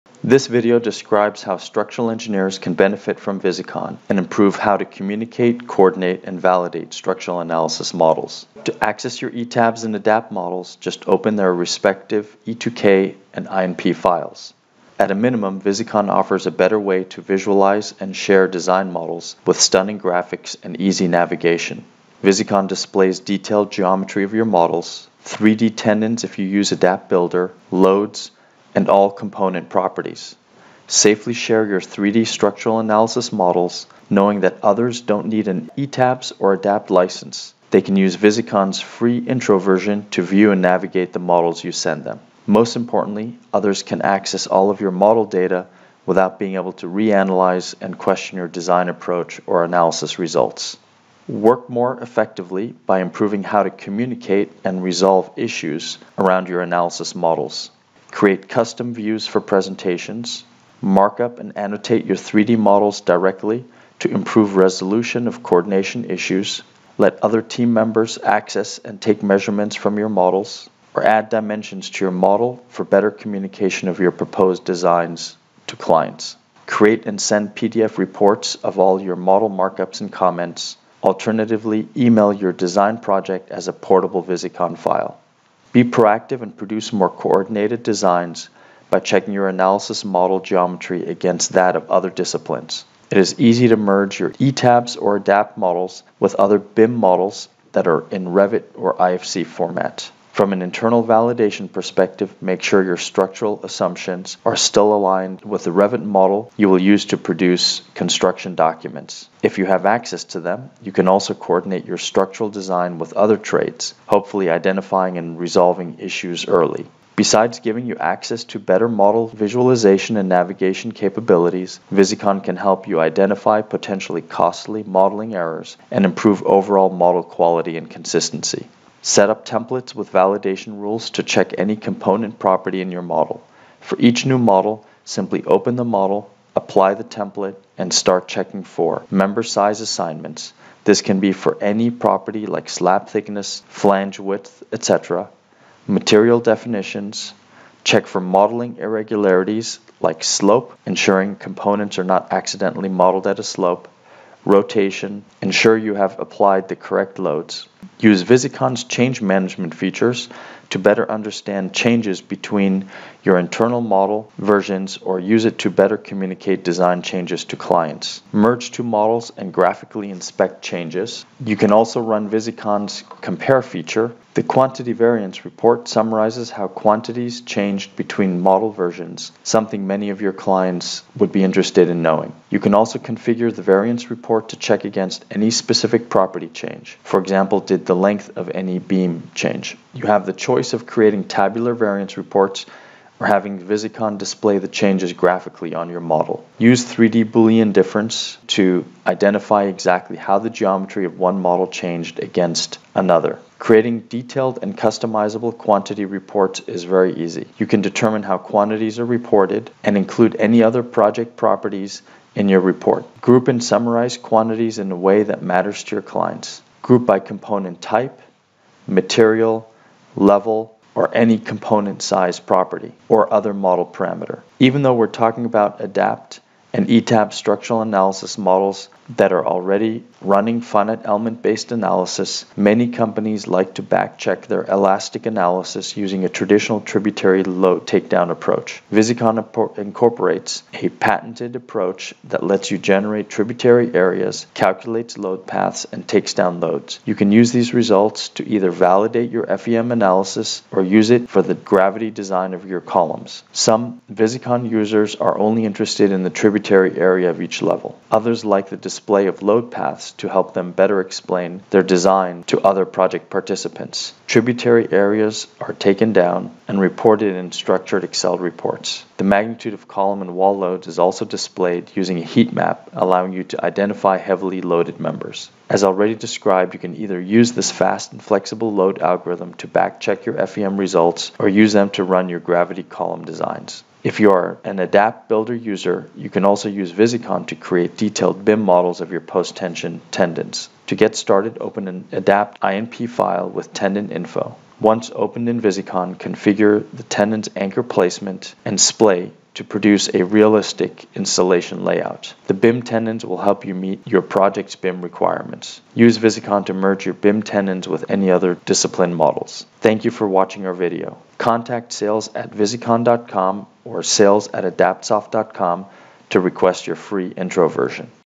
Visicon for Structural Engineers
This video is for structural engineers that use ETABS or ADAPT. It provides an overview of how Visicon can be used to improve the visualization, coordination and quality of these structural analysis models.